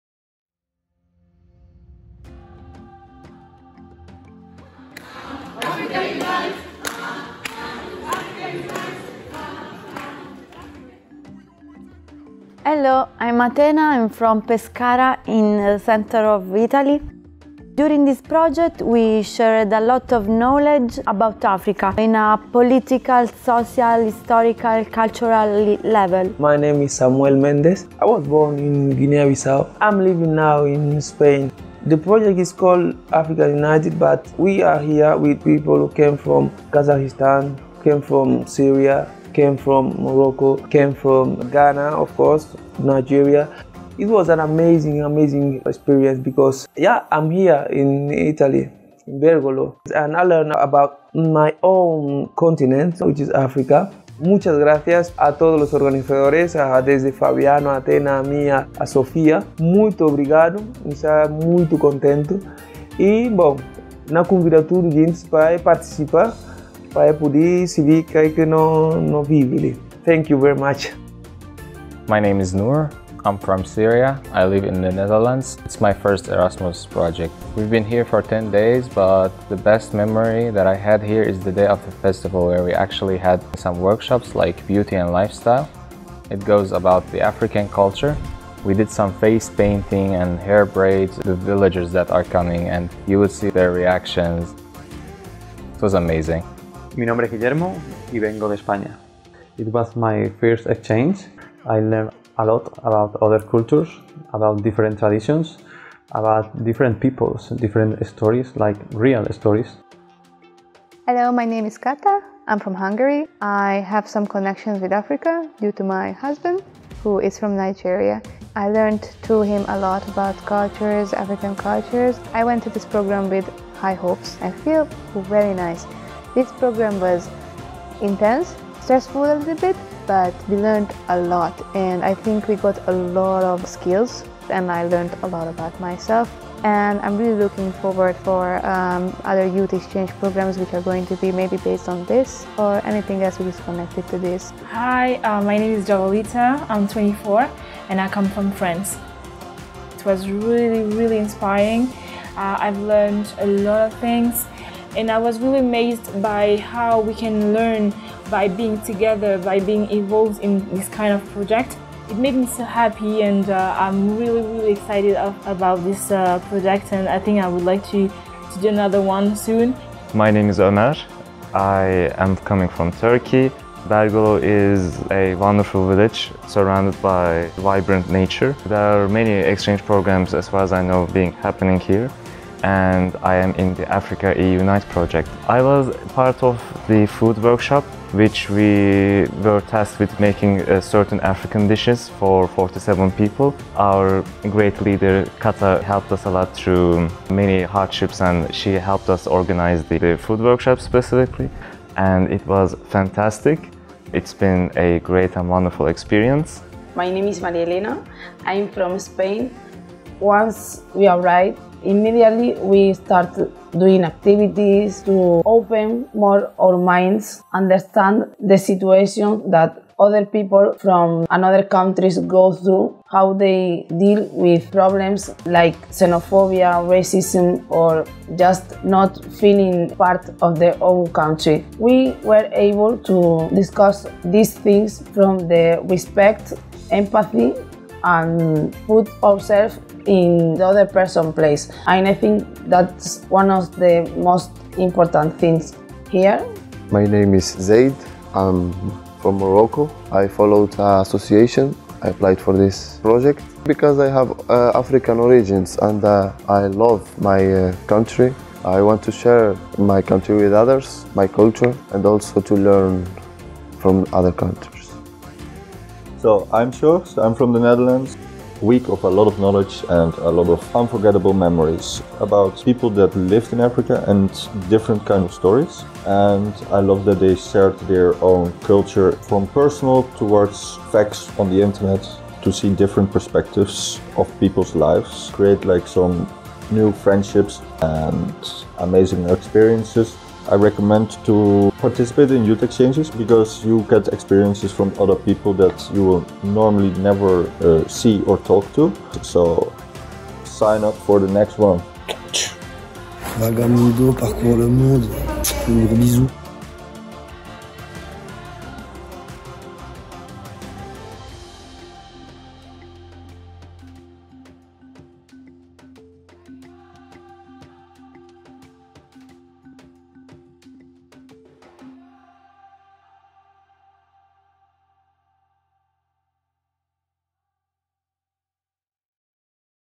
AFRICA EUNITE - Youth Exchange
Africa EUnite is a project that connects and brings together young people living in Europe who have a strong connection with Africa. The aim of the project is to learn and share upon the wide concept of African cultures, reflect on issues important for young Europeans with African roots and share about those topics with the local population during the local public fest in the village of Bergolo (Italy).  The project involved 40 young people with foreign origins, mainly from Africa, in activities aimed to rediscover together African cultural features, learn about topics related to migration and share them with the local population during the local fest of Bergolo village. The project was developed mainly through the non-formal education method and it was co-funded by the Erasmus Plus Program through Agenzia Nazionale per I Giovani.